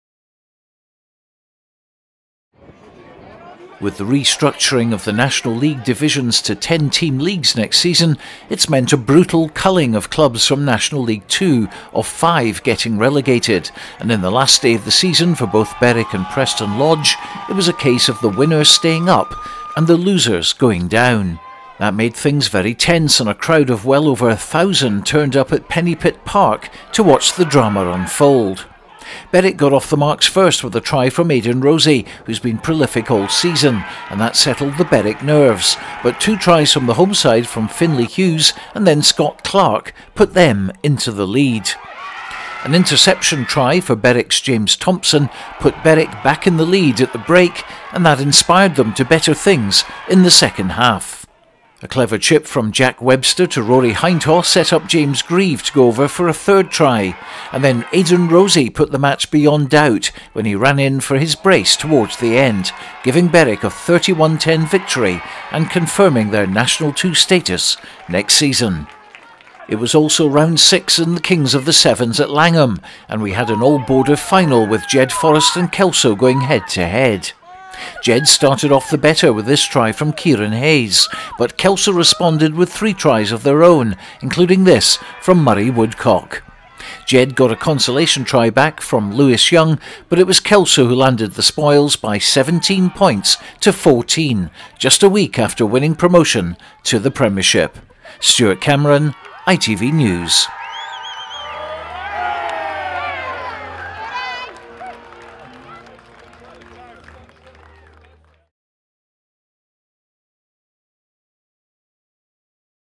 ITV BORDERS RUGBY ROUNDUP - PRESTON LODGE v BERWICK & LANGHOLM 7s - 24.4.23
Stuart Cameron reports from Pennypit as Berwick battled to stay in National 2 while at Langholm 7s there was a surprise.